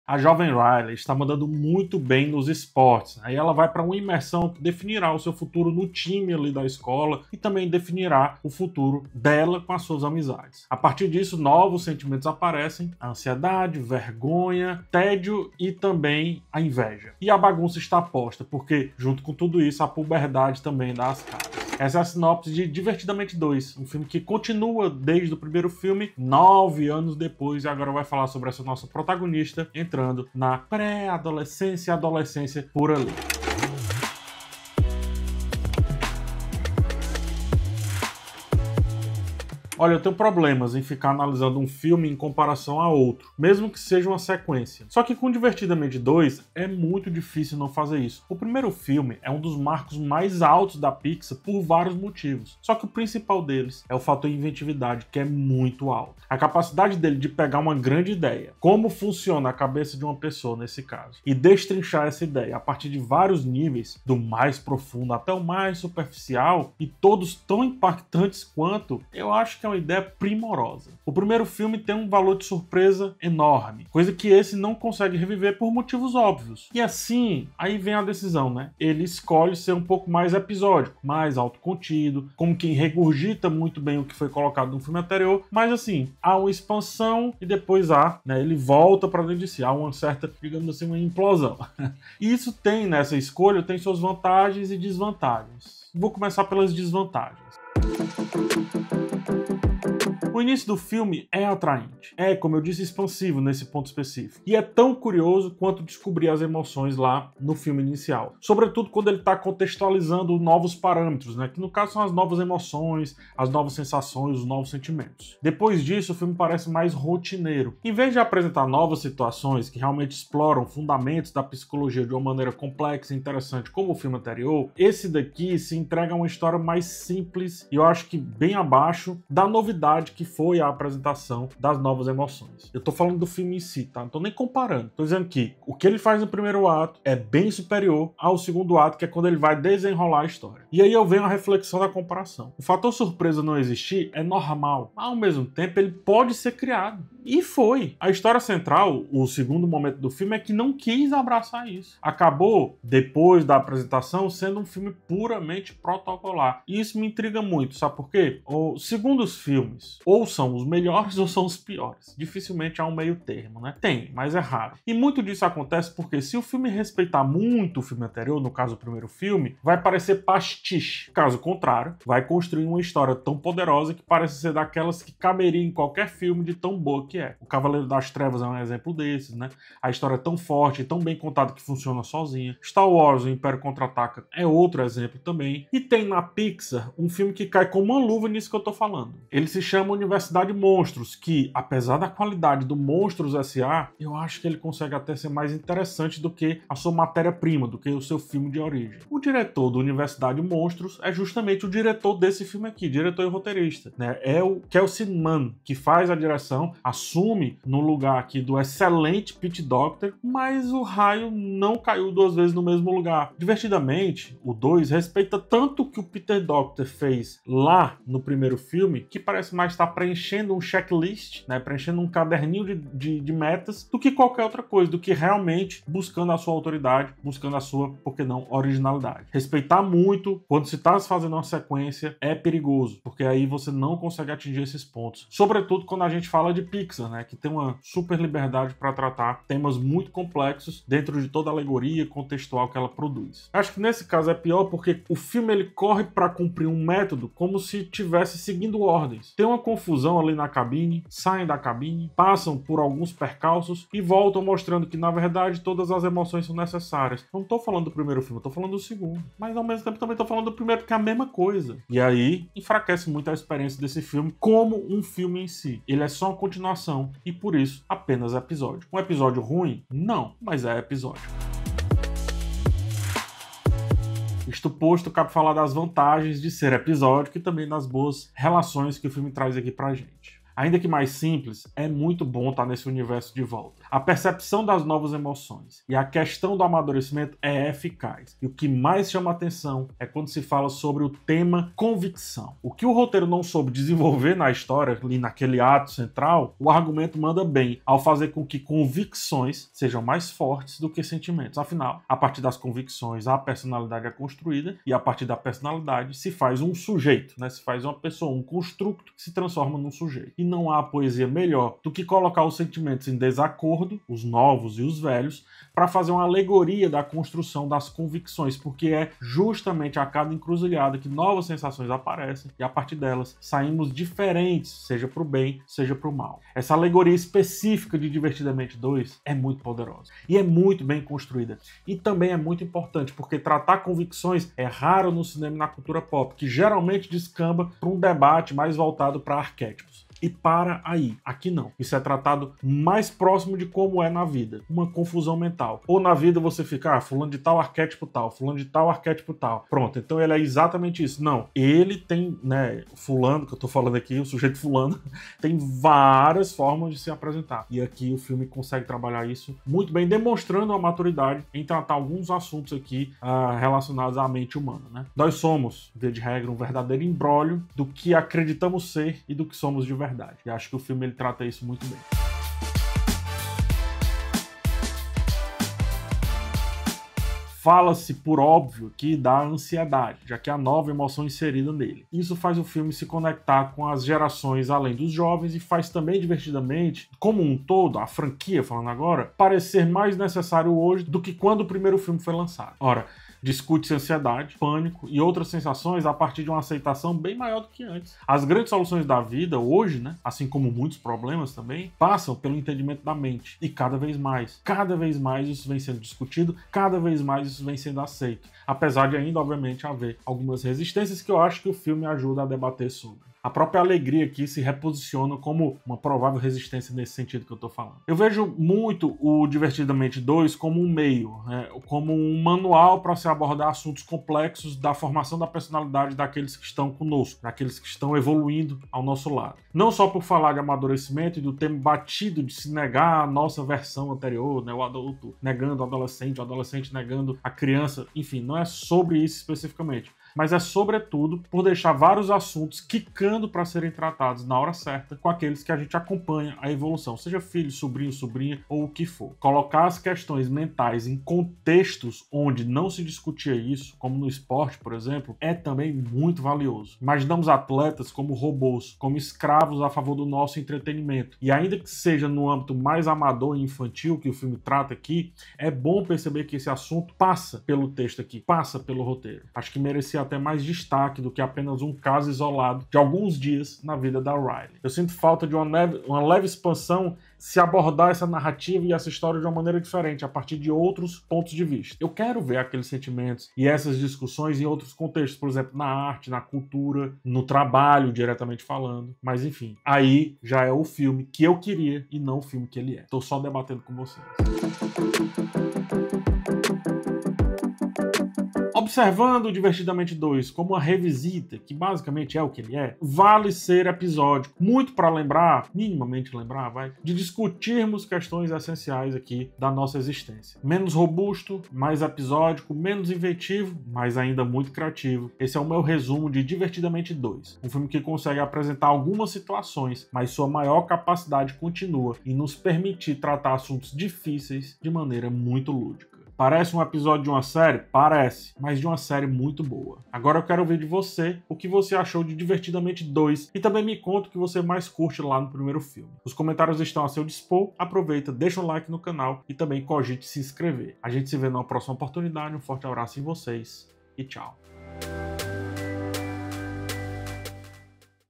Divertida Mente 2 - Episódio de uma série (boa) | Crítica do filme
Inside Out 2 (Divertida Mente 2, Divertidamente, 2024)

Riley está se destacando nos esportes e se prepara para uma imersão que moldará seu futuro no esporte e suas relações de amizade. Com isso, emergem novos sentimentos: ansiedade, vergonha, tédio e inveja. A confusão é inevitável, já que, paralelamente a esses desafios, a puberdade entra em cena. Esta é a sinopse de "Divertida Mente 2".

Kelsey Mann assume a direção substituindo Peter Docter, enquanto Meg LeFauve retorna ao roteiro em colaboração com Dave Holstein.